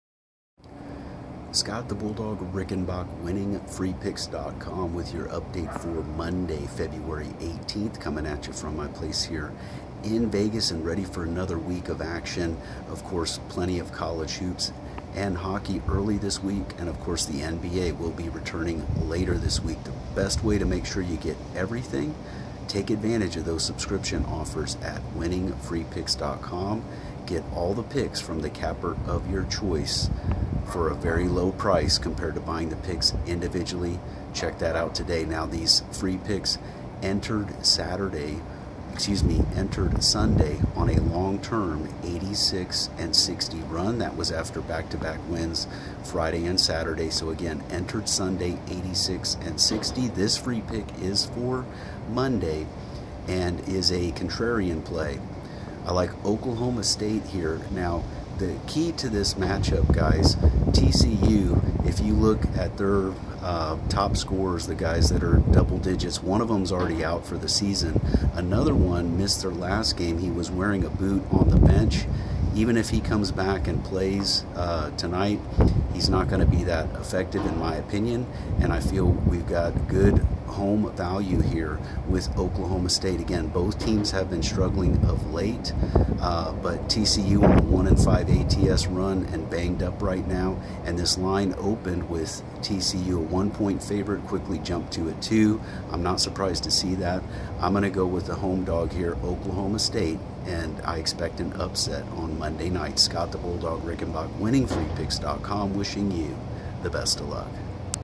NCAA Basketball Free Pick - Monday 2/18/19 - Winning Free Picks
Free College Basketball Betting Pick for Monday February 18, 2019 from Vegas resident and multi-decade Sports Handicapper Scott "The Bulldog" Rickenbach

Get free daily sports picks from the best handicappers in the world!